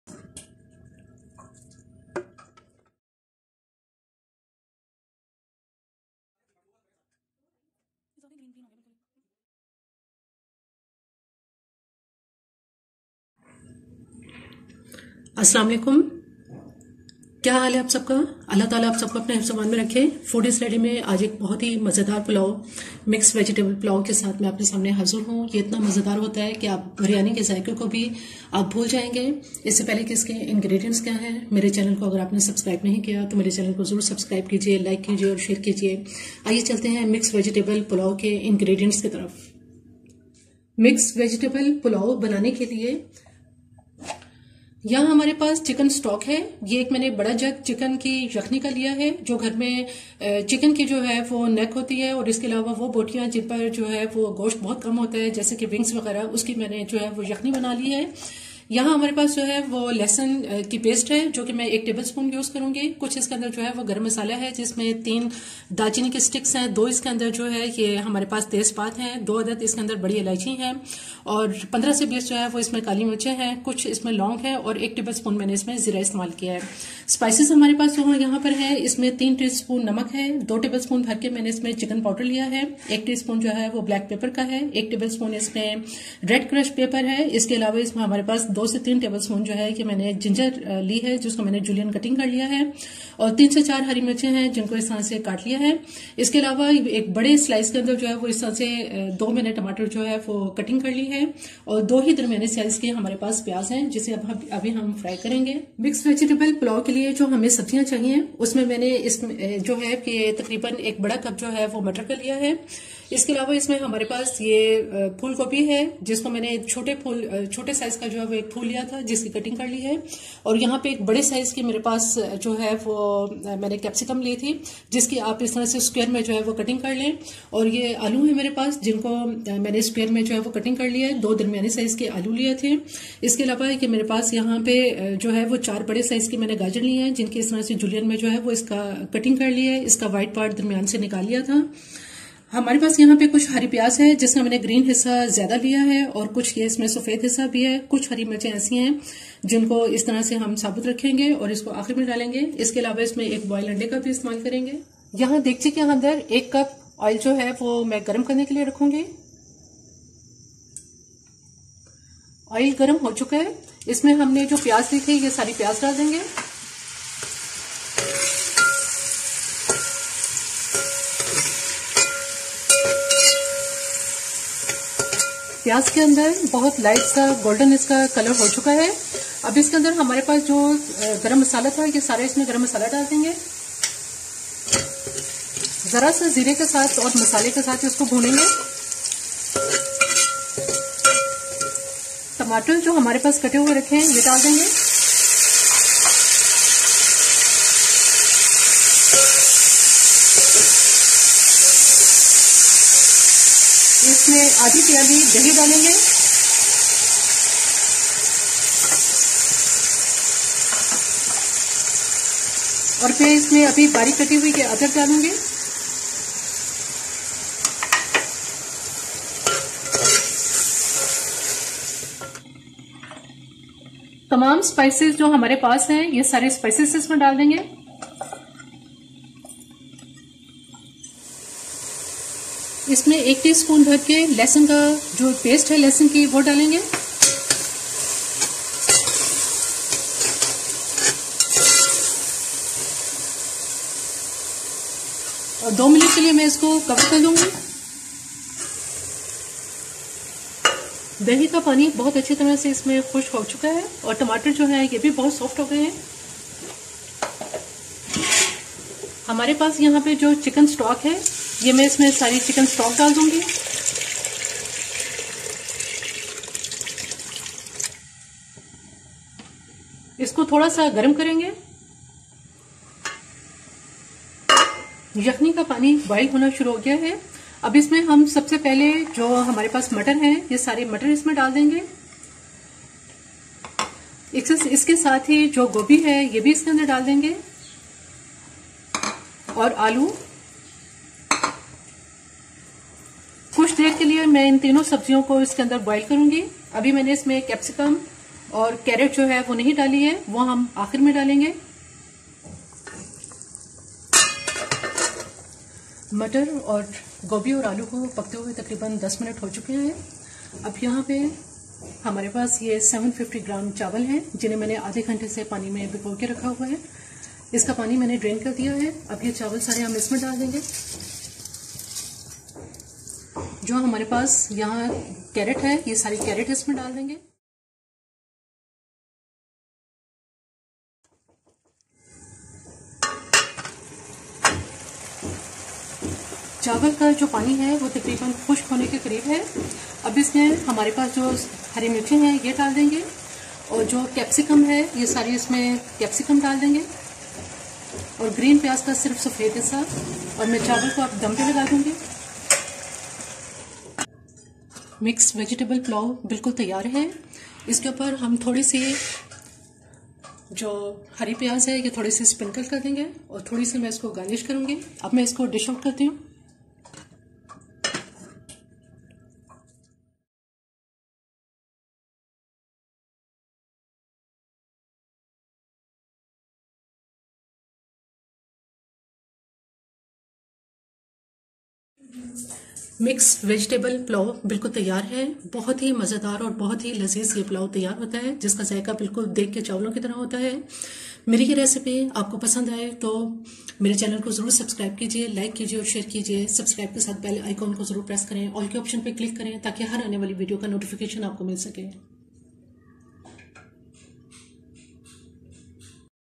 Mix vegetables pulao | sabzi pulao | mix veg pulao | Food iz ready
Assalam_o_alakum
This is the recipe of mix vegetables pulao, you can say sabzi pulao or veg pulao. I have used winter vegetables in it, you can use any seasonal vegetables which you like. Vegetables pulao is very different from biryani or chicken pulao. It is very tasty and delicious recipe of veg pulao. Its ingredients are as follows :
Chicken stock, Cinemon stick, bay leaf, black cardamom, black pepper, cloves, cumin, garlic paste, salt, chicken powder, crushed black pepper, crushed red pepper, ginger, green chillies, tomatoes, oil, onion, rice and boiled egg